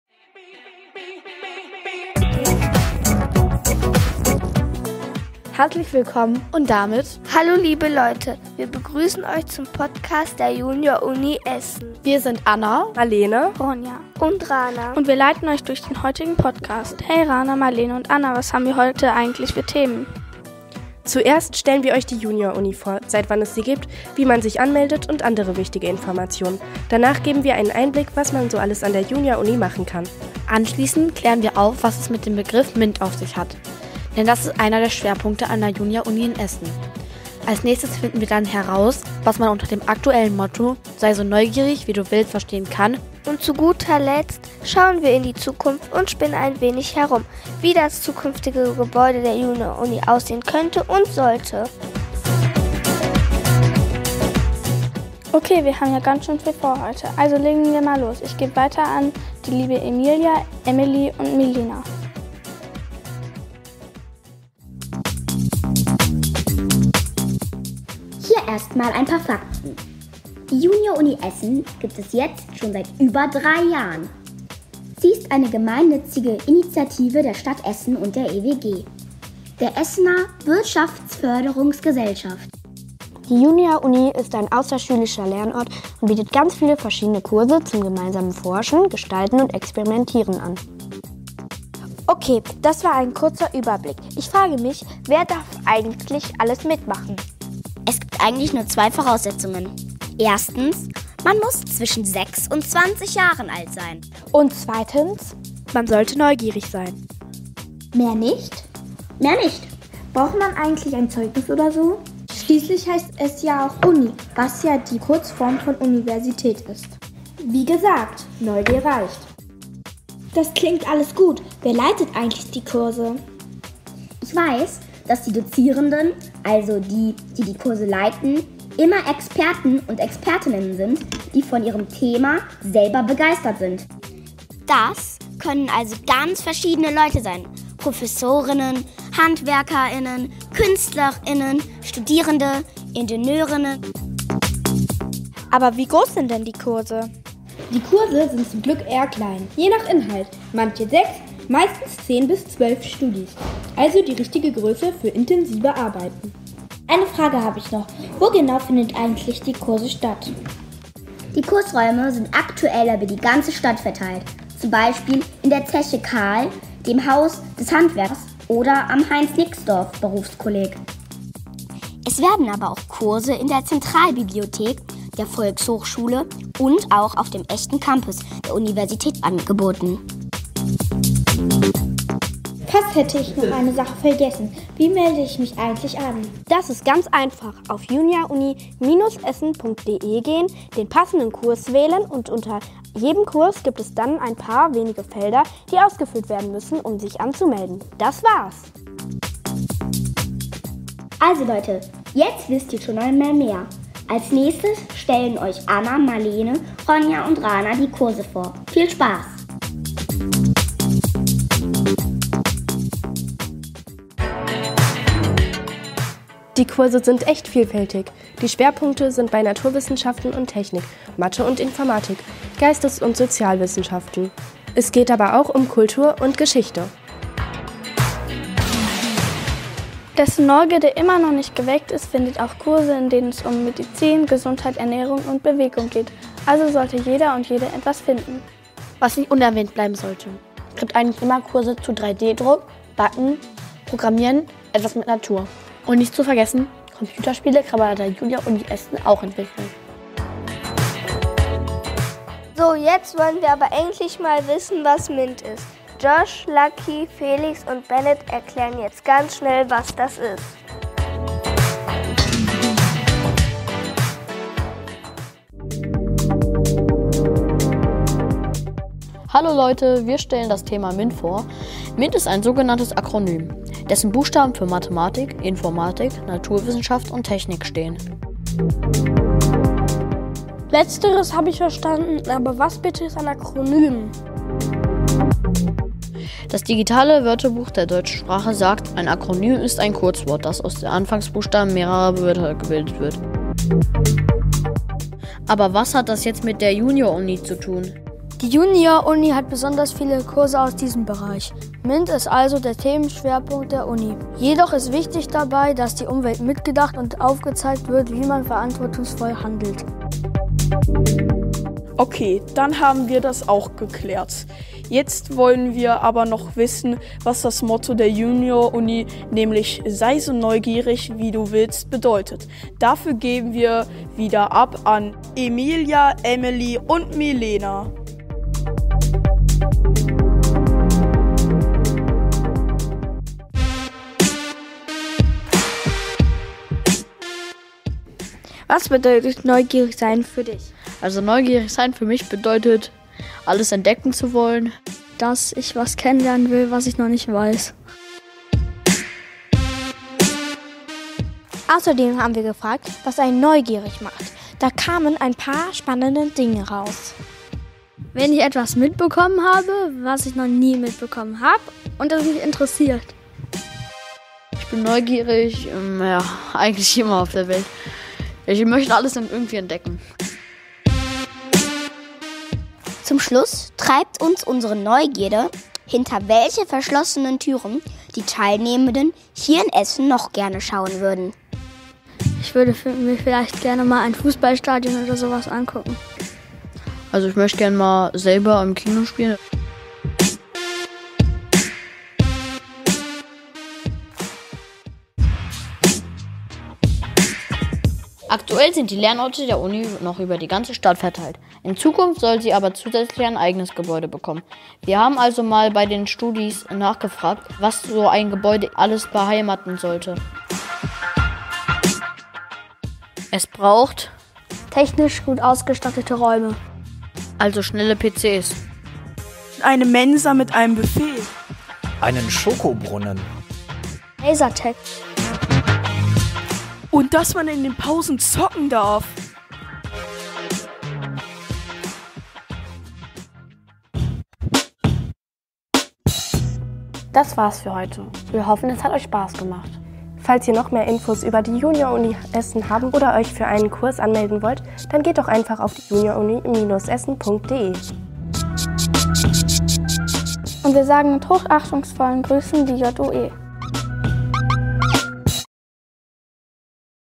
„Schritt für Schritt zum eigenen Podcast“ – Der Junior Uni Essen Podcast von und für Studis
An der Junior Uni Essen hatten Jugendliche zwischen 10 und 14 Jahren die einzigartige Gelegenheit, in die Welt der digitalen Audioformate einzutauchen. Unter dem Titel “Schritt für Schritt zum eigenen Podcast” erforschten die Studis mit unserem Dozenten Marc von den MedienMonstern nicht nur die Theorie hinter dem Medium, sondern setzten auch ihr erlerntes Wissen praktisch um.

Wir sind megastolz auf unsere Studis und dieses tolle Ergebnis...
Was meinst du? Höre selbst.

Wir freuen uns über neue Studis, Dozierende und Kooperationen.
Du möchtest mehr über uns erfahren, dann besuche gerne unsere Homepage. Hier findest du alle Informationen: